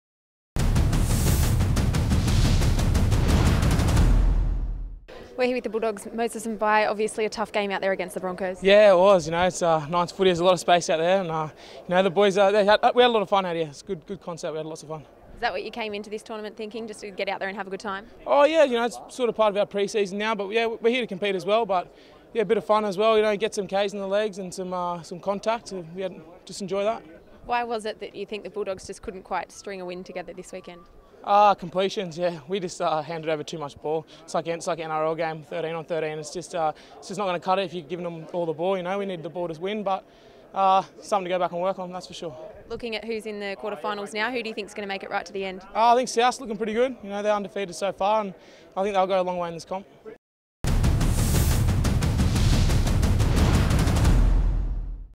Day 2 - Auckland Nines Game 24 post match: Mbye
Auckland Nines post match interview with Moses Mbye